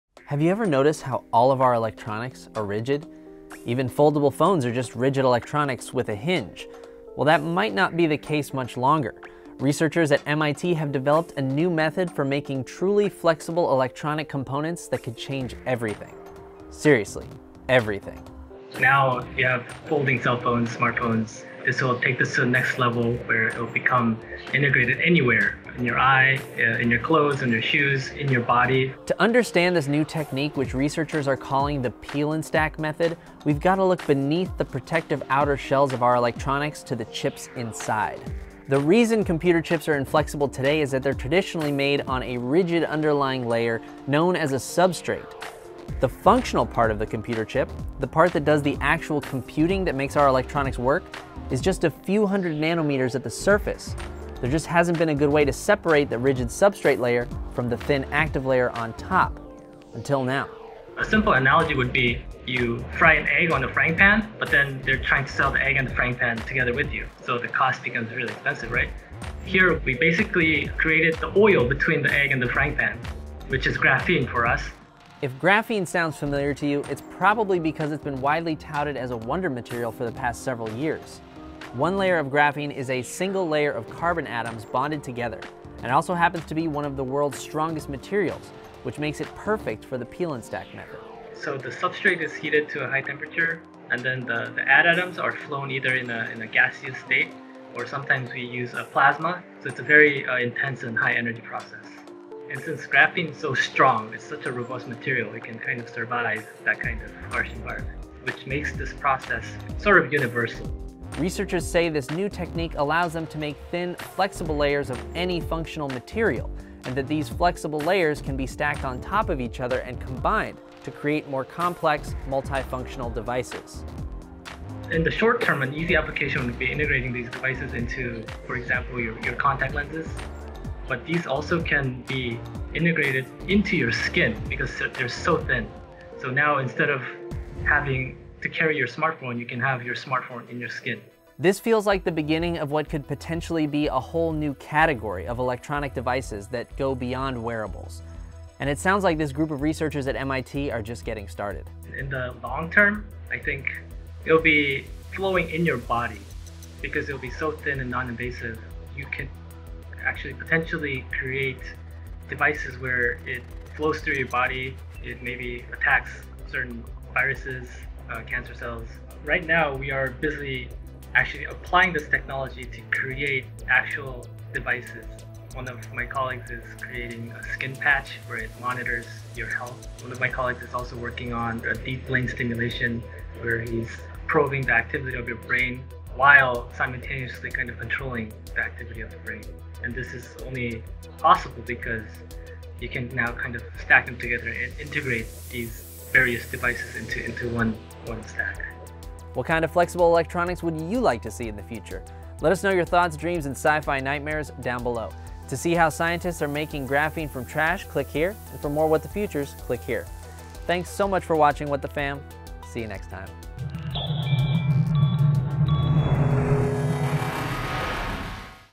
Truly flexible electronics are coming for your skin, eyes, and body
Researchers at MIT have developed a new method for fabricating electronic components that opens up a world of possibilities, including flexible, stackable, semitransparent and noninvasive devices. They could someday be integrated into our clothes, shoes, contact lenses and even our brains! It's been called "remote epitaxy" and the "peel and stack" method, and it involves the use of the wonder material known as graphene.